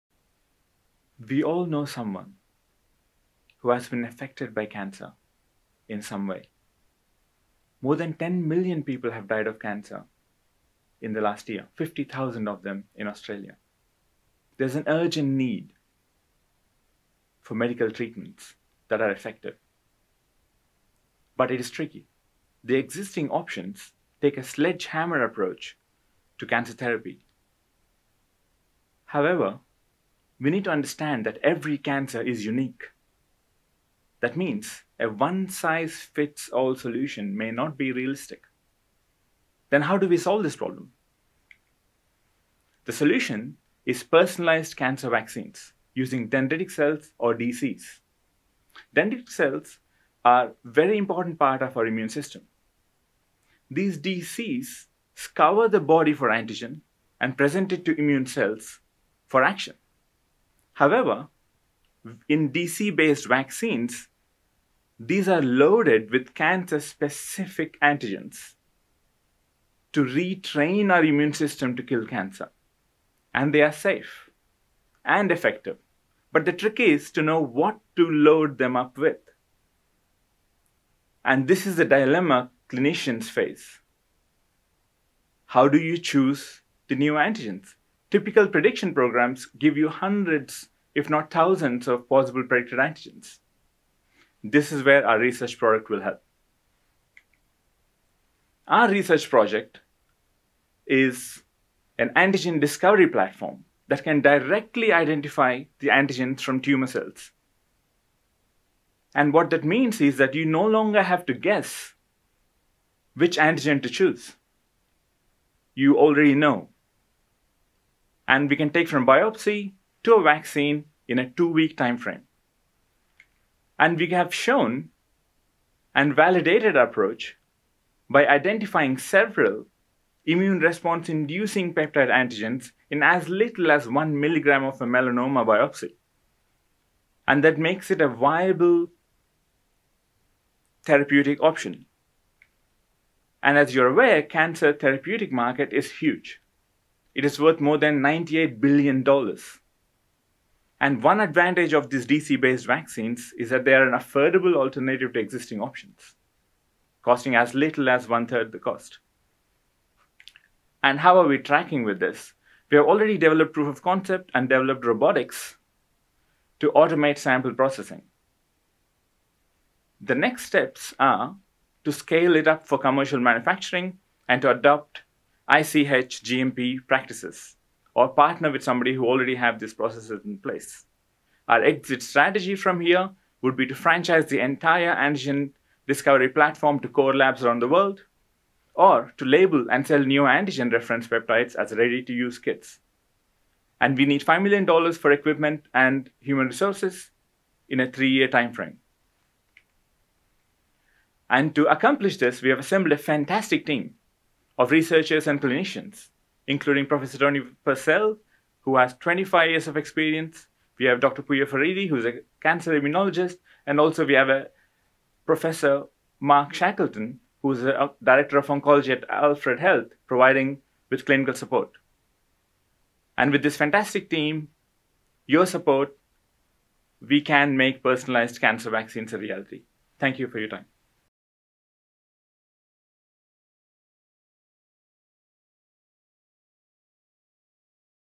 Dr Ramarathinam: developing personalised cancer vaccines
Dr Sri Ramarathinam presents on using dendritic cells to potentially develop novel personalised cancer vaccine technology. His presentation was prepared as a pitch to industry for the Monash BDI’s “So you think you can pitch” competition. Dr Ramarathinam took out second place – awarded by a panel of industry judges - in the competition.